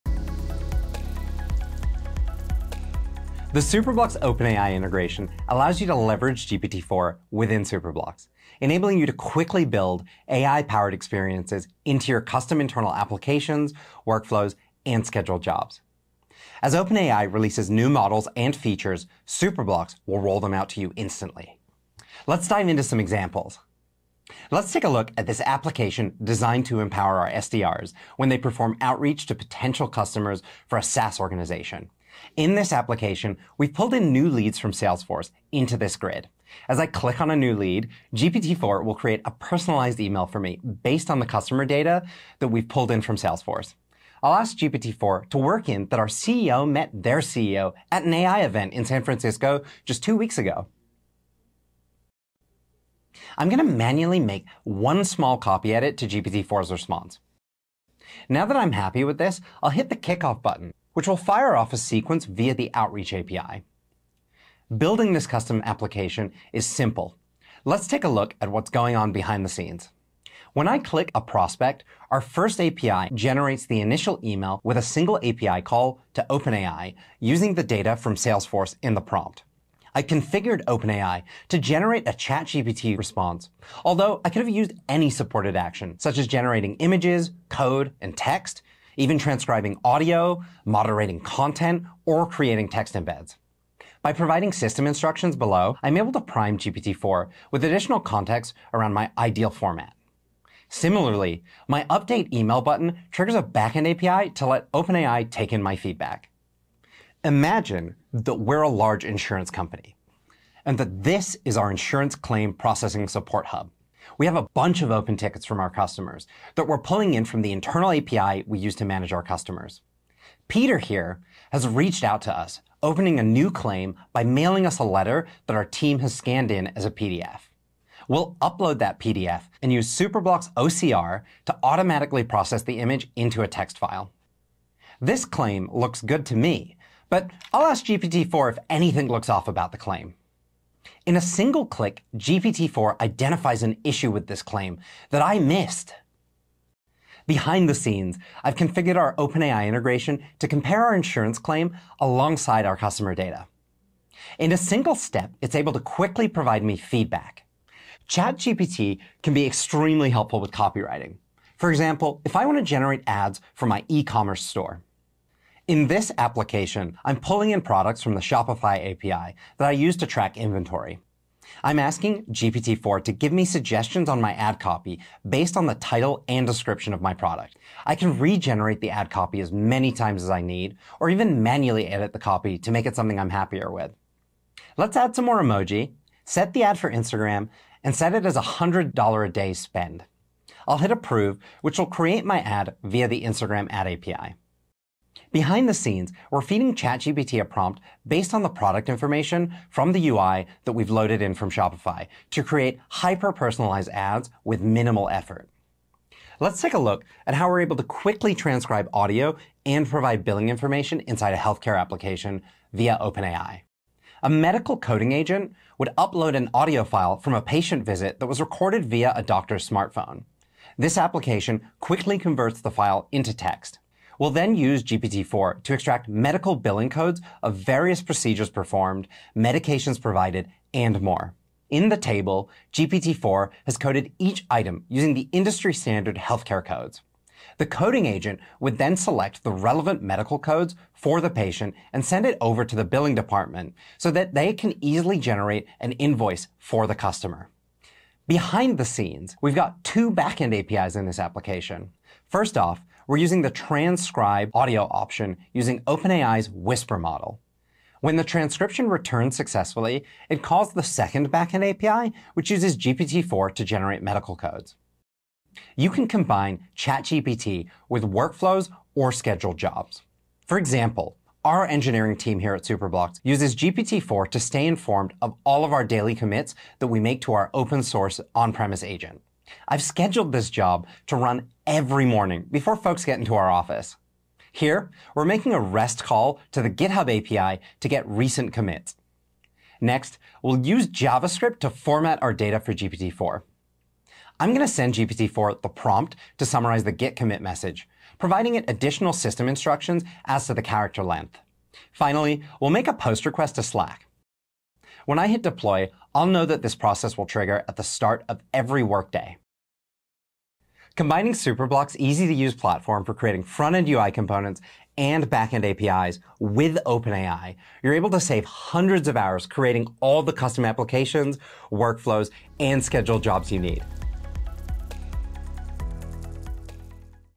AI-Powered Applications with OpenAI
Use the OpenAI integration within Superblocks to generate text or code based on a text prompt you provide it. Provide AI capabilities to end-users or leverage it within APIs to enrich your Superblocks APIs. Some examples include flagging fraudulent activities based on customer transaction history, suggesting potential fixes to customer issue tickets for support reps, or generating product descriptions for e-commerce products based on a text prompt.